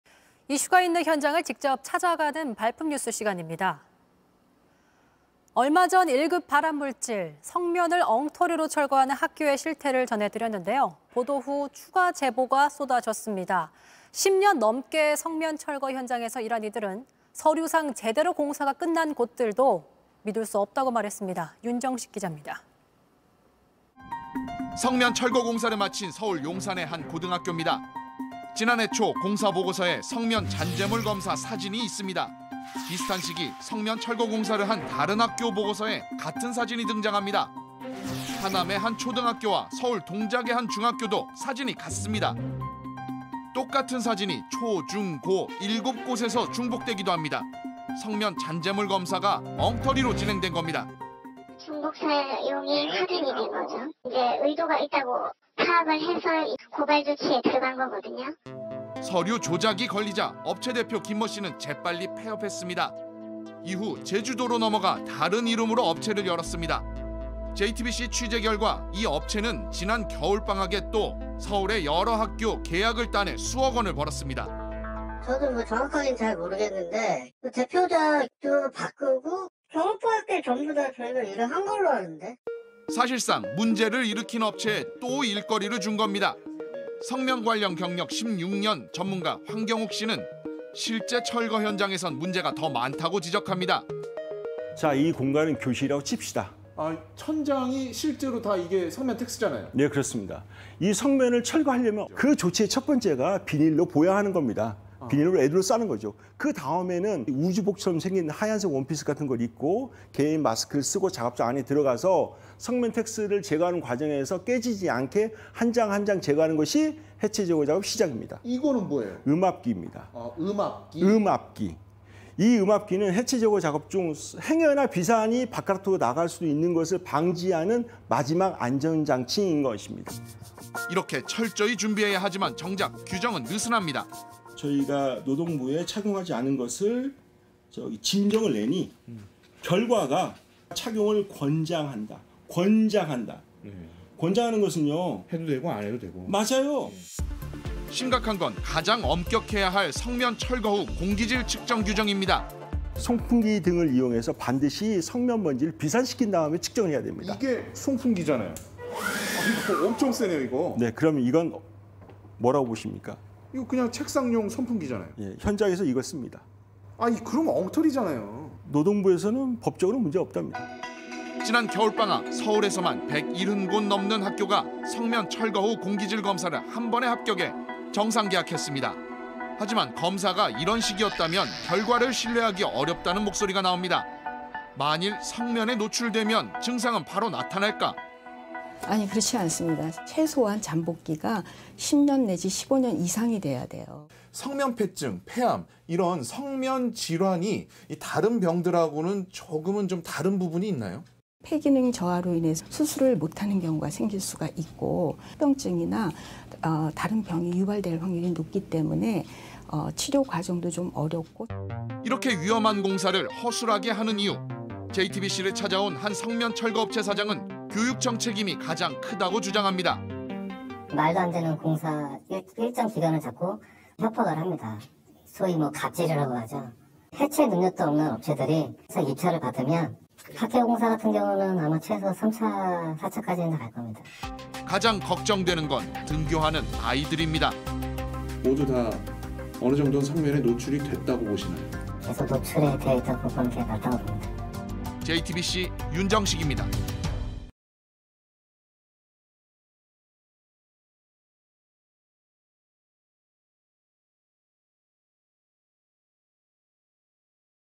[발품뉴스] 애들 교실에 '엉터리 석면 검사'…걸려도 이름 바꿔 수억 벌어 / JTBC 뉴스룸
이슈가 있는 현장을 직접 찾아가는 '발품뉴스' 시간입니다. 얼마 전 1급 발암물질, 석면을 엉터리로 철거하는 학교의 실태를 전해드렸는데요. 보도 후 추가 제보가 쏟아졌습니다. 10년 넘게 석면 철거 현장에서 일한 이들은 서류상 제대로 공사가 끝난 곳들도 믿을 수 없다고 말했습니다.